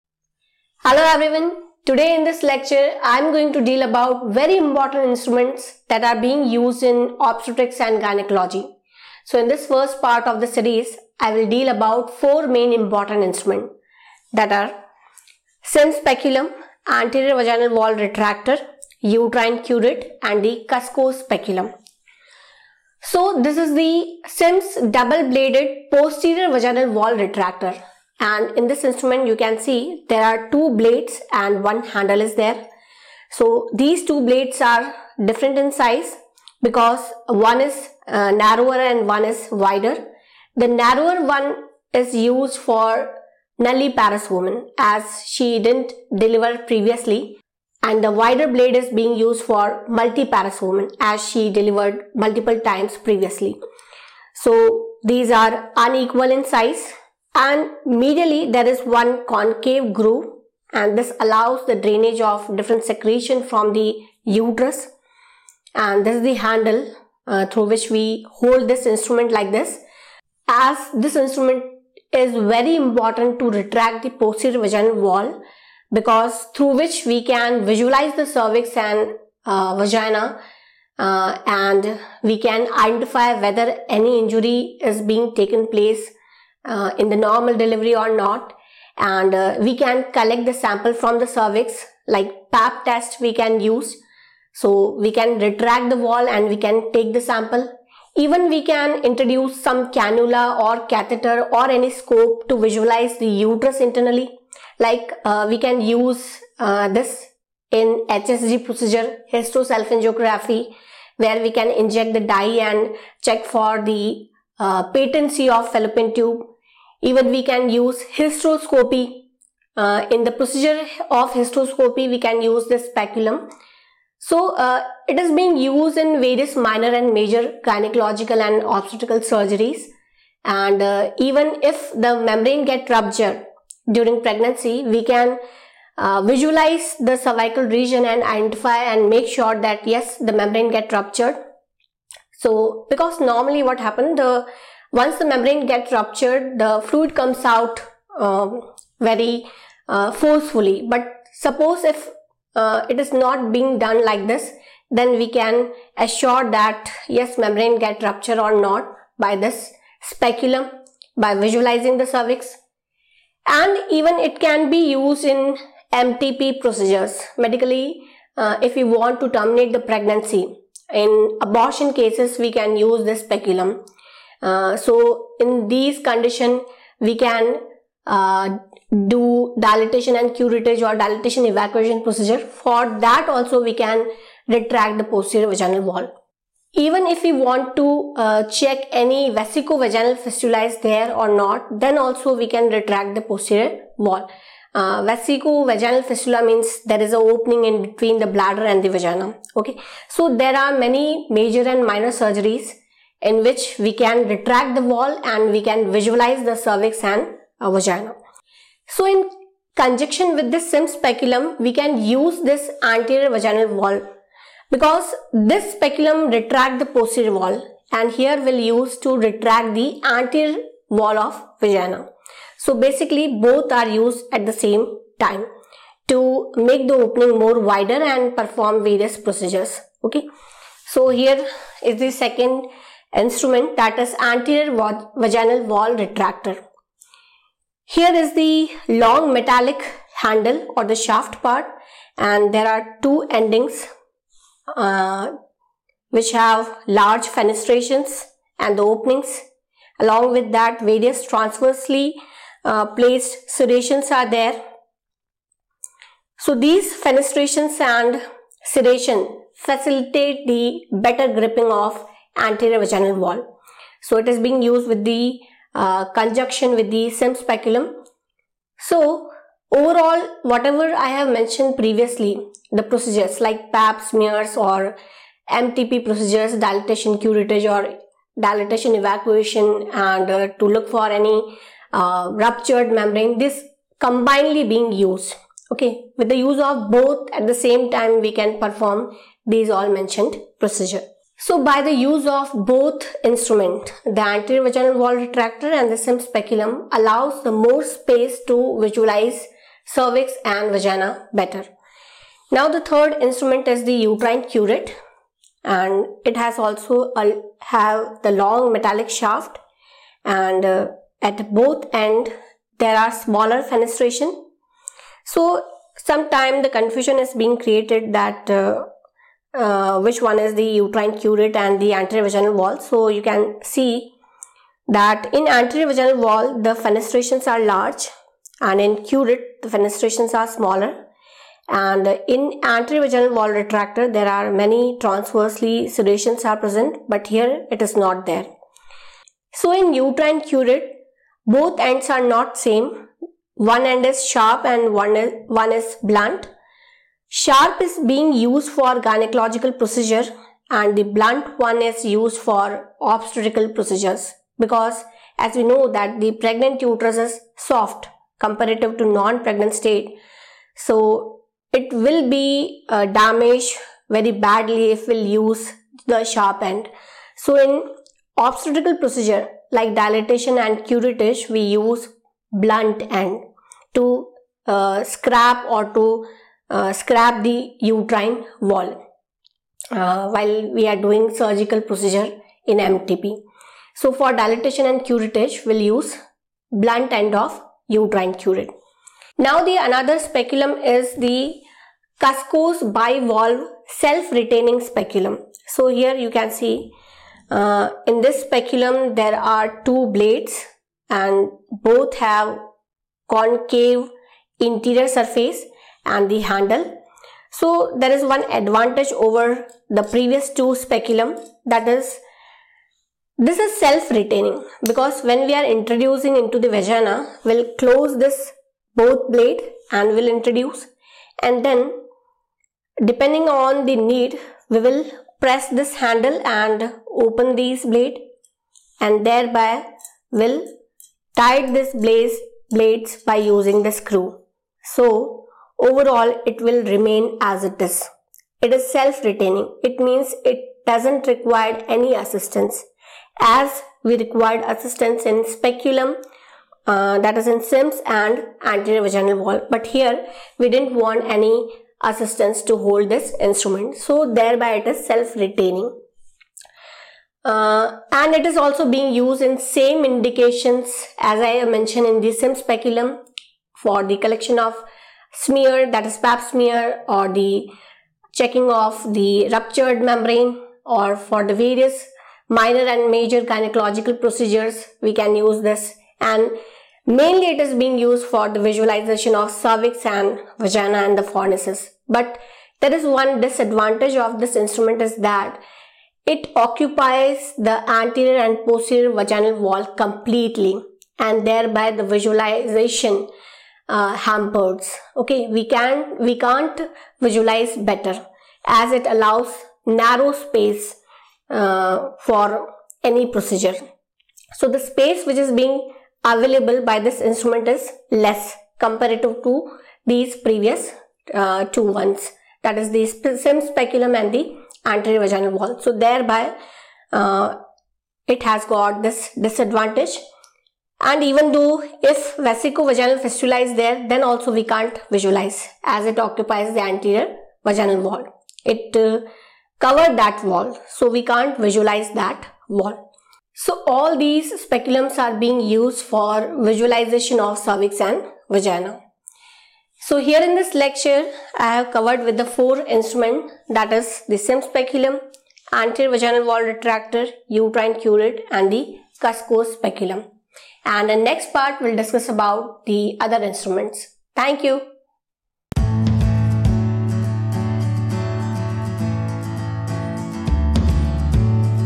OBG Instruments Part -1 (English) | Nursing Lecture
Here very important practically asked instruments are explained -
- Sim's Speculum
- Cusco's Speculum
- Anterior Vaginal Wall Retractor
- Uterine Curette

00:00 Instruments in this part
00:25 Sim's Speculum
03:52 Anterior Vaginal Wall Retractor
05:03 Cusco's Speculum
06:31 Uterine Curette